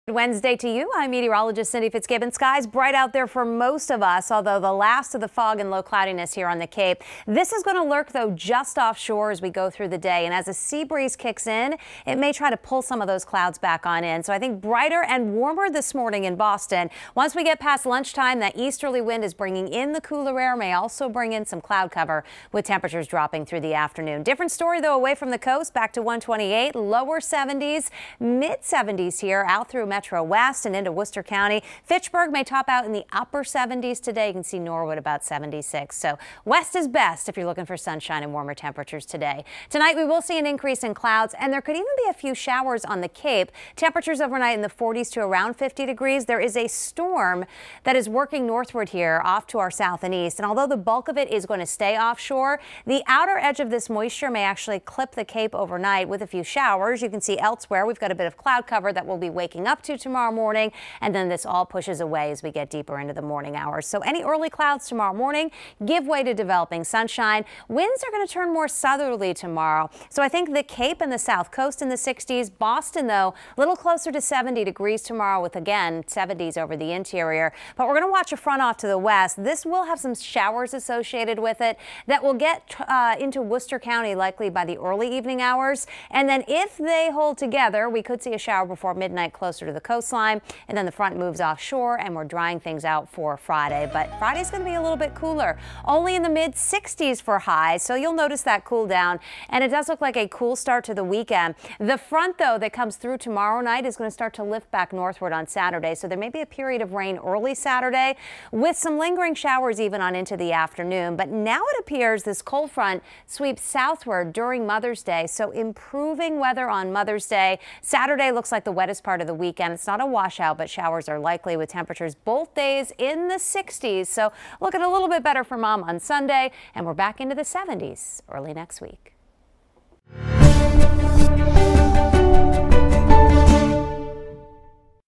Video: Warmer, more sun west
Temps will be in the 60s along the coast but warmer further west.